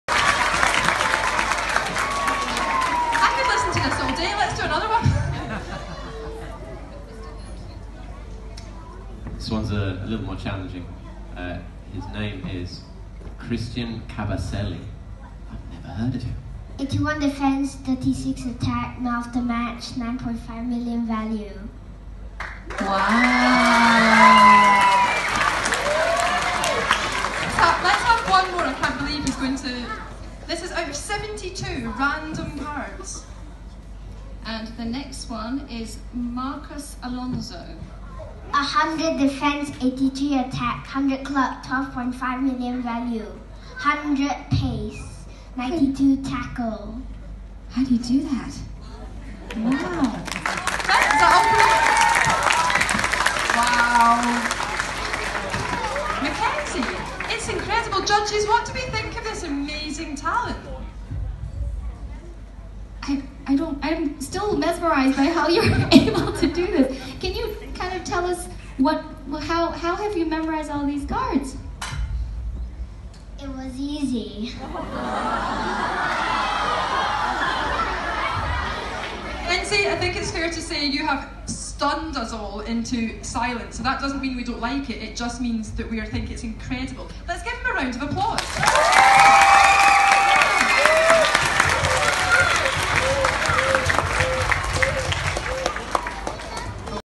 My School Talent Show Memorize Football Cards @ Part 2
My first talent show in school is to remember 72 football cards information. The principal and teachers are the ones who give questions and I need to let them know. It's an amazing experience!